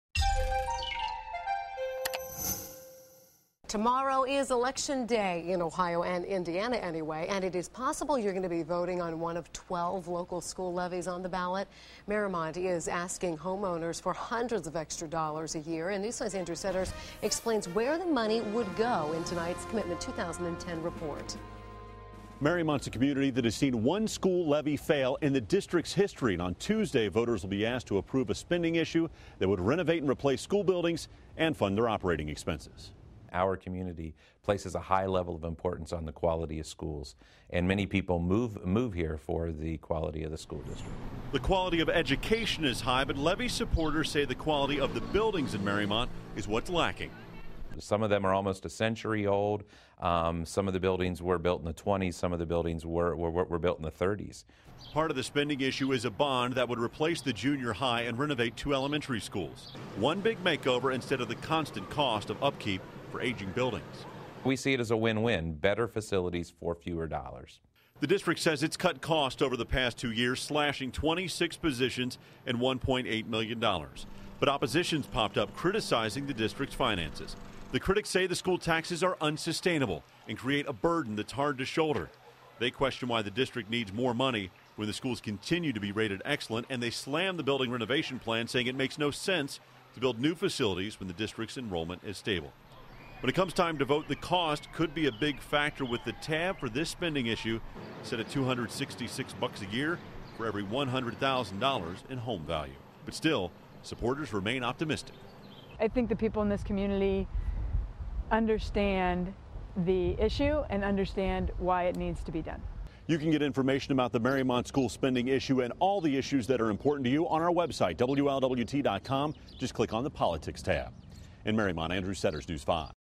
Mariemont Schools Ask Voters To Approve Levy
Andrew Setters reports on the school levy that Mariemont is asking voters to approve.